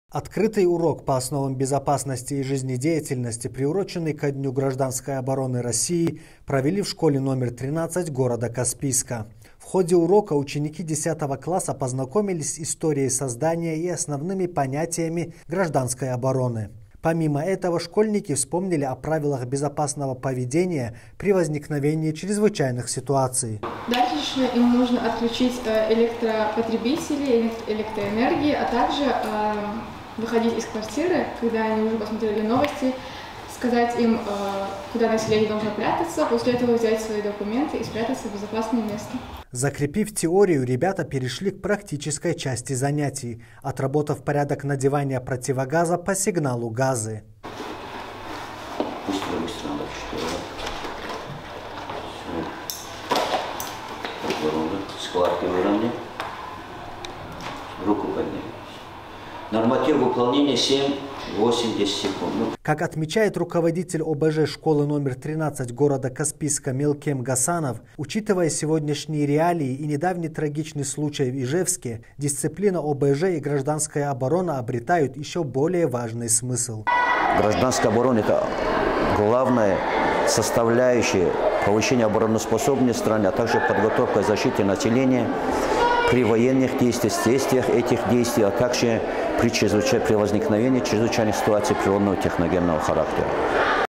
Открытый урок по ОБЖ ко Дню гражданской обороны РФ
Открытый урок по основам безопасности и жизнедеятельности, приуроченный ко Дню гражданской обороны России, провели в школе №13 г. Каспийска.

В ходе урока ученики 10-го класса познакомились с историей создания и основными понятиями гражданской обороны. Помимо этого, школьники вспомнили о правилах безопасного поведения при возникновении чрезвычайных ситуаций.